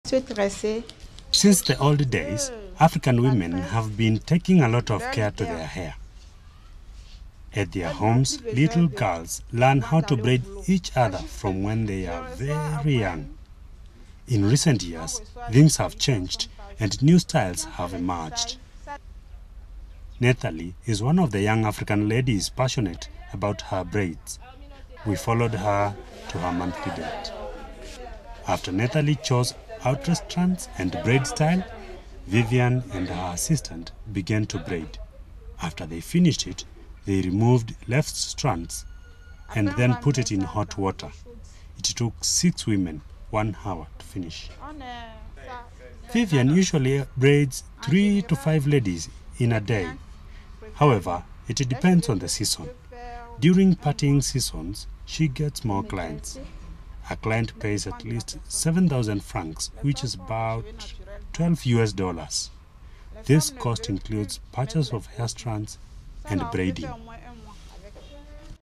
African hair braiding: a re-fashioned tradition
Since the old days, African women have been taking a lot of care to their hair.

At their home, little girls learn how to braid each other when they are very young.

In recent years things have changed and new styles have emerged